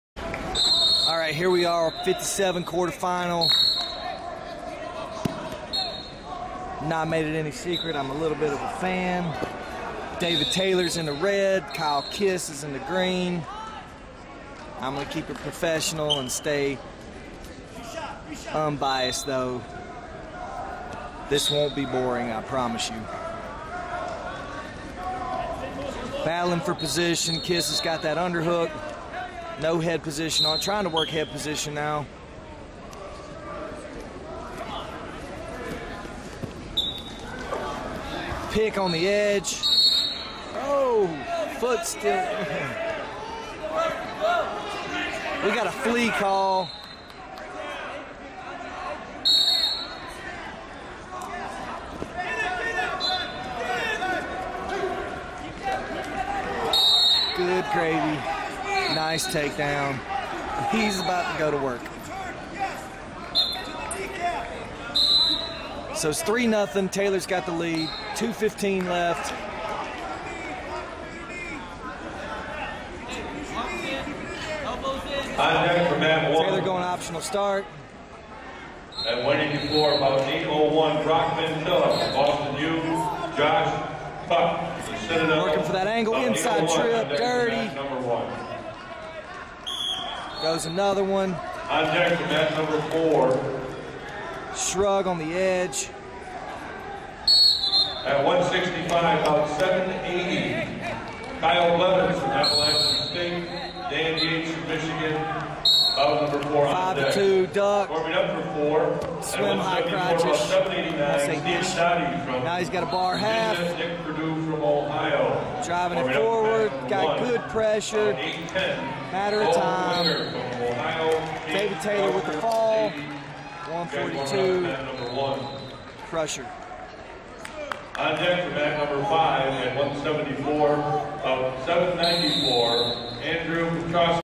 157 lbs quarter finals David Taylor Penn State vs Kyle Kiss North Carolina
The Southern Scuffle was and has remained one of the premier regular season events in college wrestling. After UNCG dropped their wrestling program UTC took over this prestigious event and it continues to bring in some of the top talent and match ups in the history of the sport.